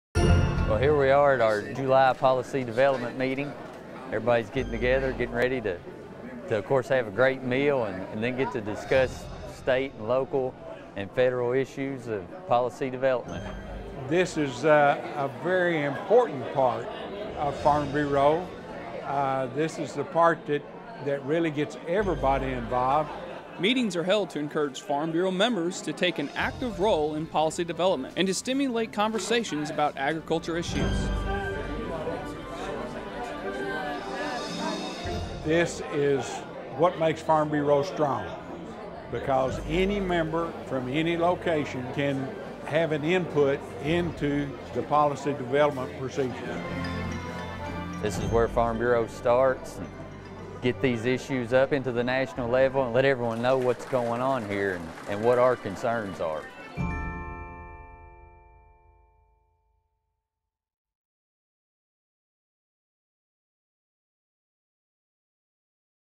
Kentucky Farm Bureau Minute Policy Development
On today's Farm Bureau Minute, we visit two district meetings that are held to encourage farm bureau members to take an active role in developing policy at the grassroots level.